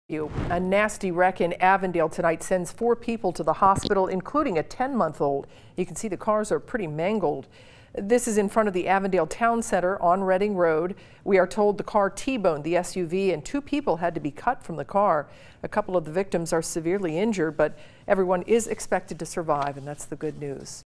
Police say excessive speed, impairment involved in Avondale crash
AVONDALE, Ohio (WKRC) - Police say excessive speed and impairment led to a crash in Avondale involving a small child Thursday evening, December 29.

Police say the driver of a Ford Escape was driving north on Reading Road, near Lexington, when she lost control, went left of center and struck a Nissan Maxima head-on. The Escape rolled onto its side.

The Escape's driver is in good condition at Good Samaritan Hospital. The Maxima's driver and a passenger were taken to UC Medical Center, where they're expected to recover. A toddler in the Maxima was checked out at Children's Hospital.

So far, no charges have been filed.